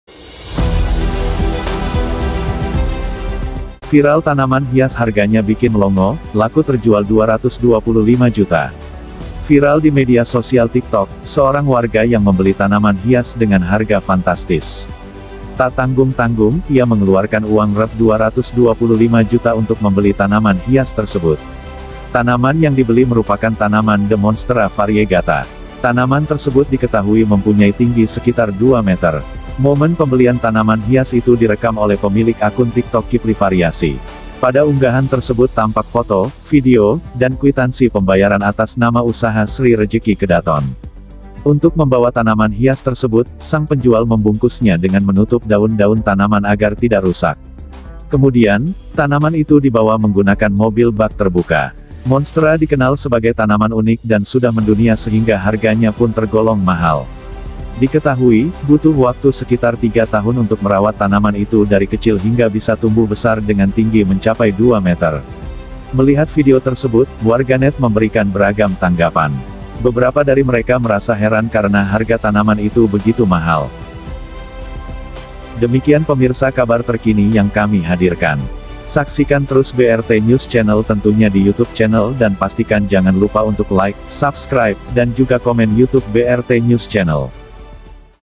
Viral Tanaman Hias Harganya Bikin Melongo, Laku Terjual Rp 225 Juta
Tak tanggung-tanggung, ia mengeluarkan uang Rp 225 juta untuk membeli tanaman hias tersebut.

Tanaman yang dibeli merupakan tanaman The Monstera Variegata.

Tanaman tersebut diketahui mempunyai tinggi sekitar 2 meter.

Momen pembelian tanaman hias itu direkam oleh pemilik akun TikTok kiplivariasi,

Pada unggahan tersebut tampak foto, video, dan kwitansi pembayaran atas nama usaha Sri Rejeki Kedaton.

Lebih lengkapnya silahkan tonton video nya sampai selesai.

Demikian pemirsa kabar terkini yang kami hadirkan.

Terima Kasih.